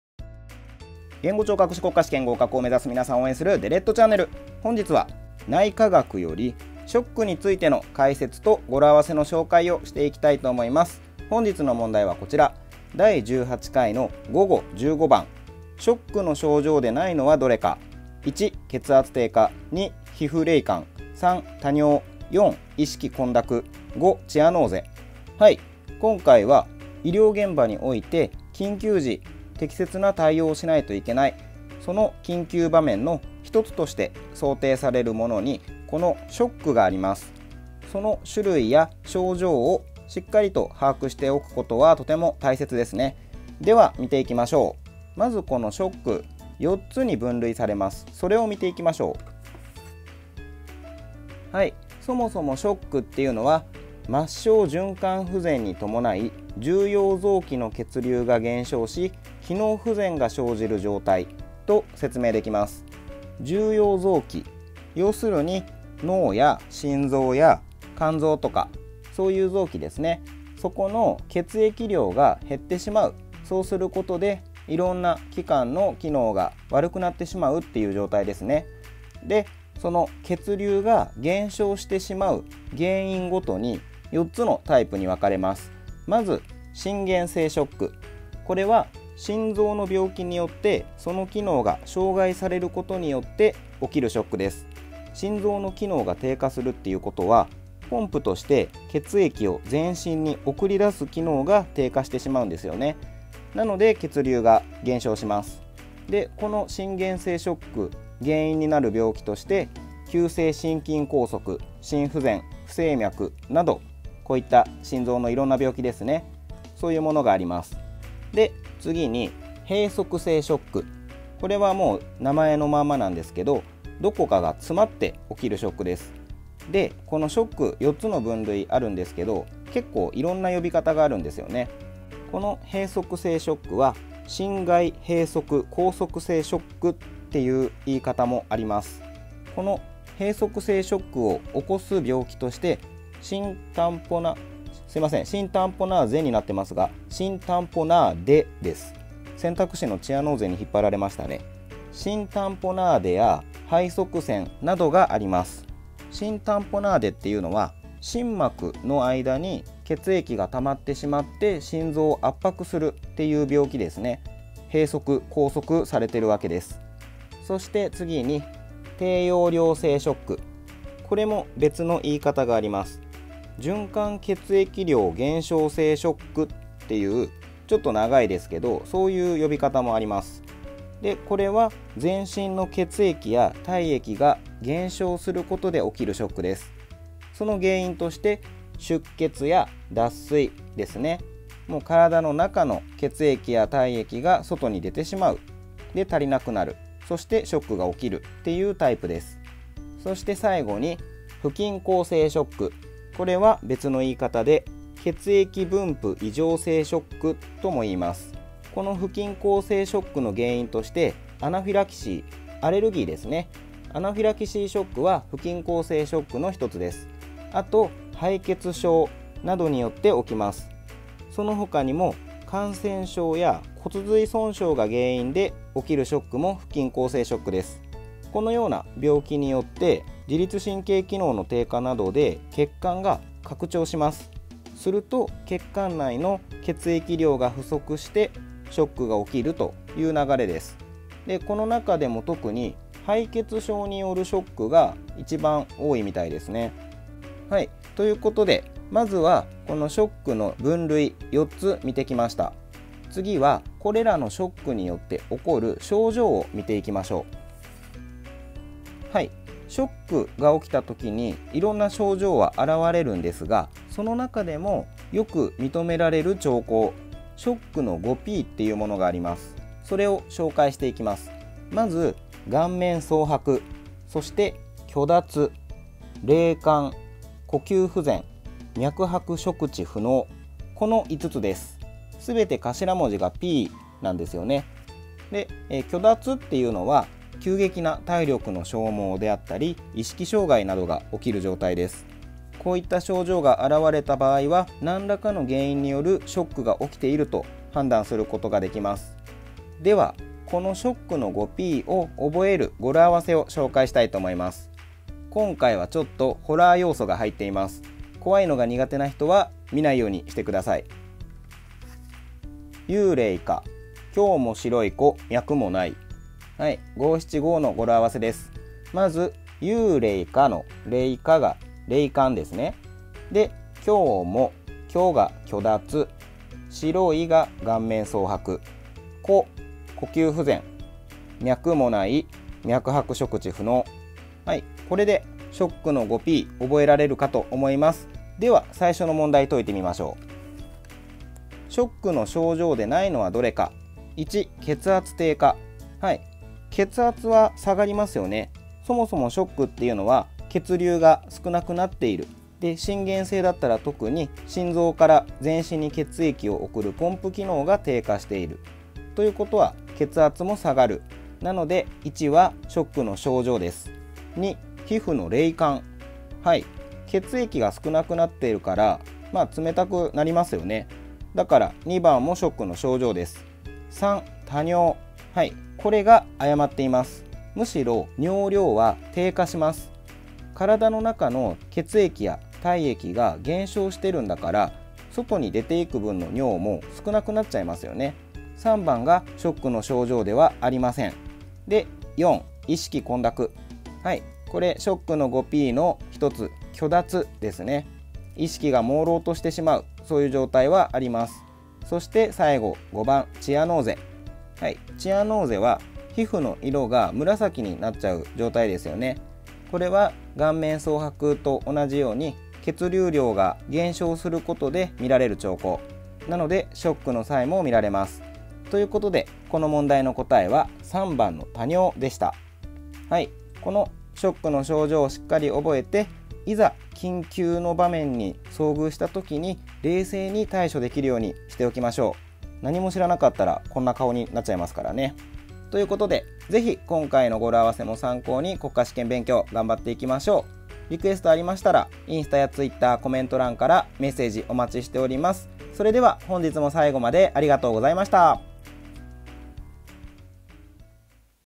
【語呂合わせ】ショック！！！！
敗血症で起きるショックは、顔面は白じゃなくて赤くなるらしいです。